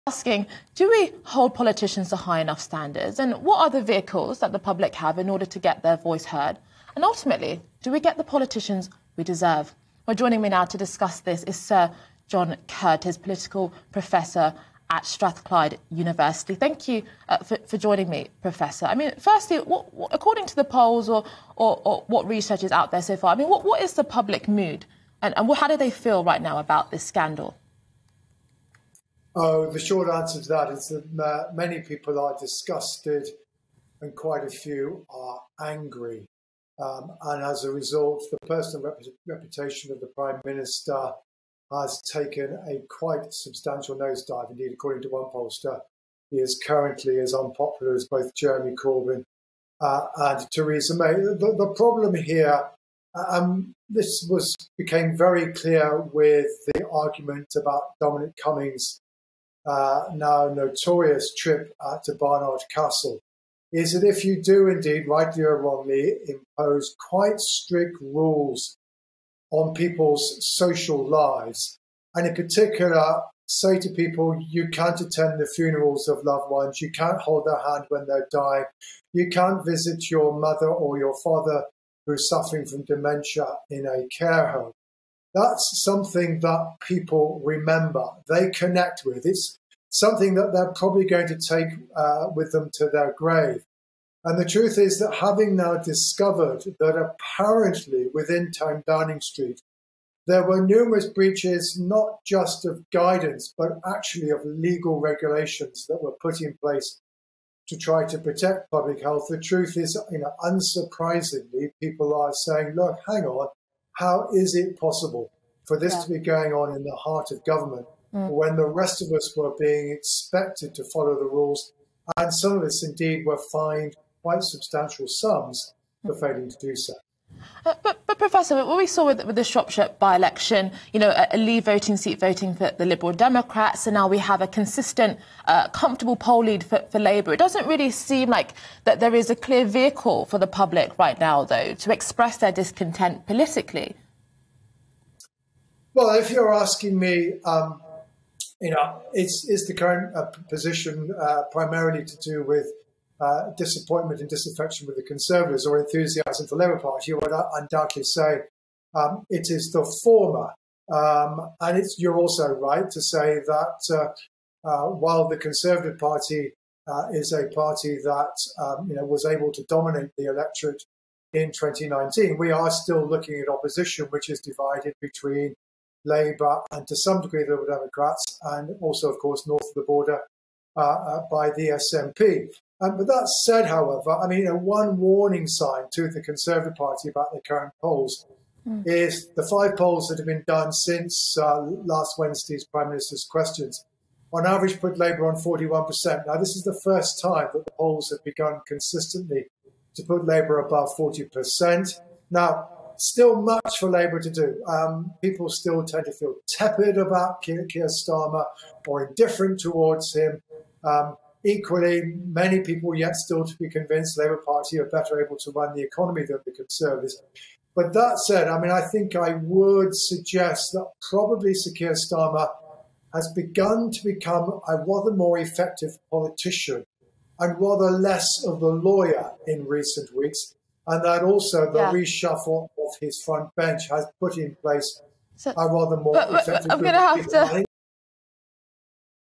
John Curtice says Labour still have 'much to do' despite positive polling
‘This is the first time that the polls have begun consistently putting Labour above 40%’

John Curtice says recent polling looks positive for Labour, but warns they still have ‘much to do.’

Watch GB News: Freeview 236, Sky 515, Virgin 626